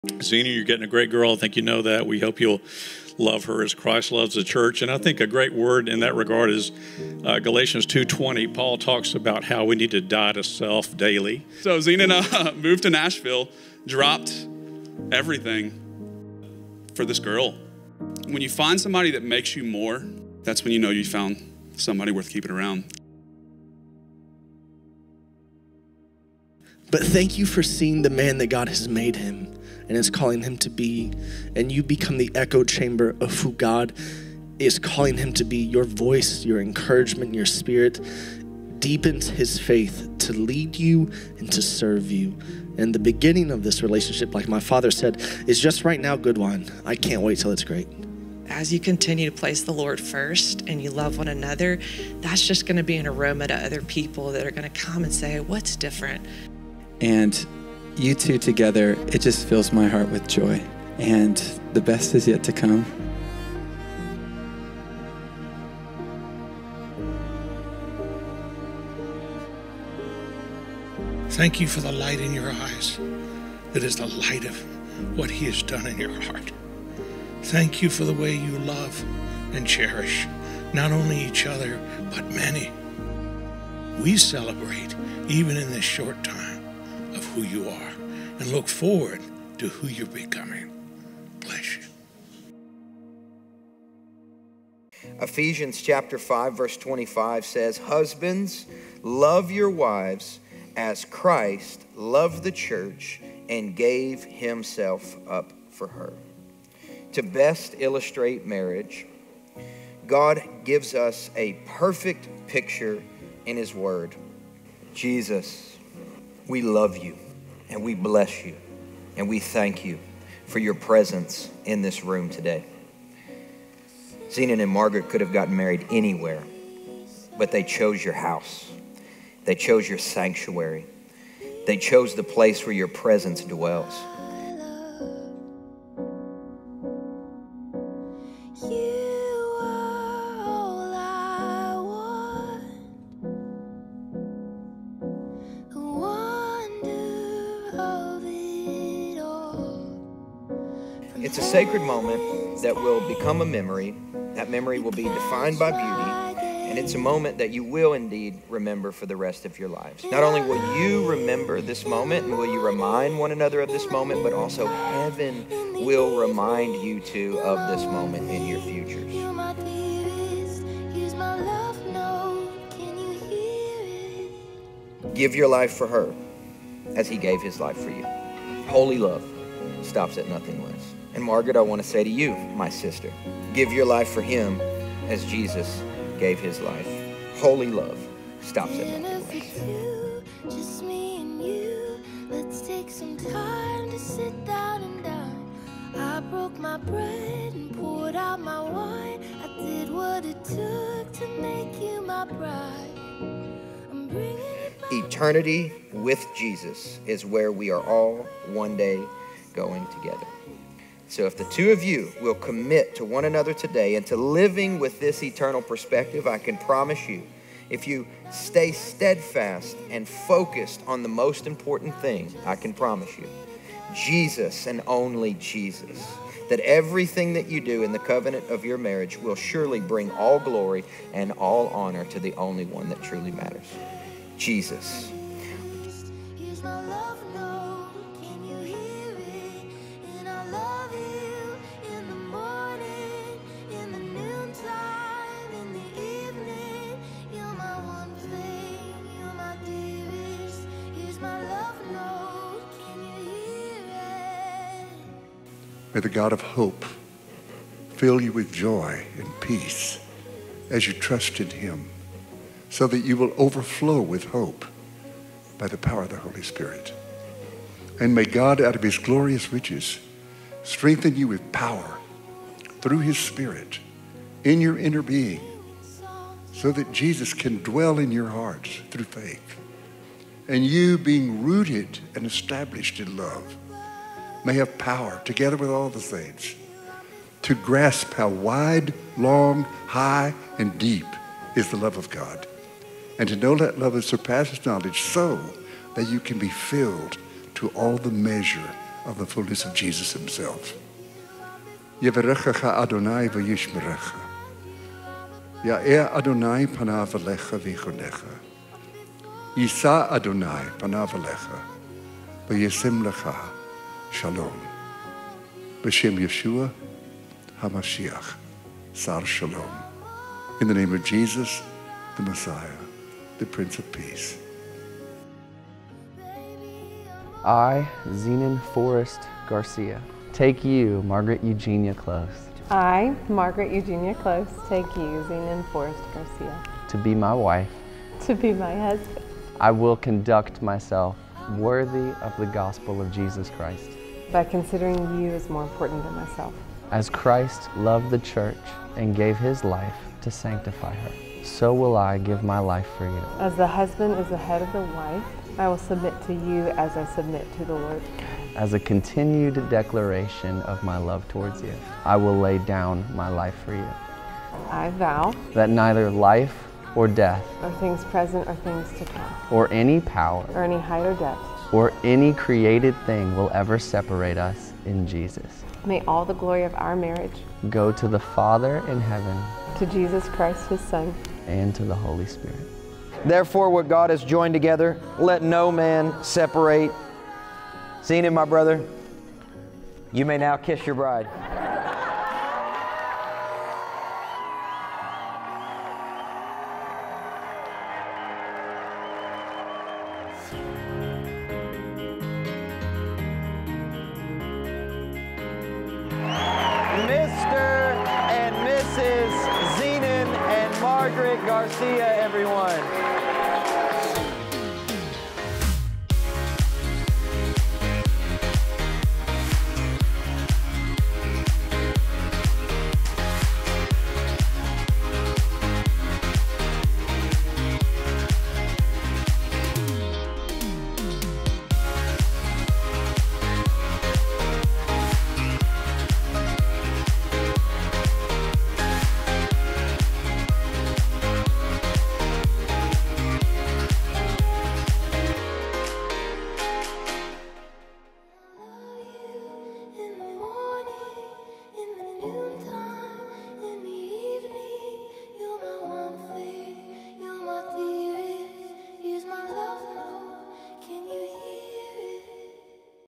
Garcia Wedding 2022
David and Ali Young with "120 Weddings" produced a gorgeous overview of our rehearsal worship night and wedding.  We are thrilled with how they captured the heart and spirit of our celebration.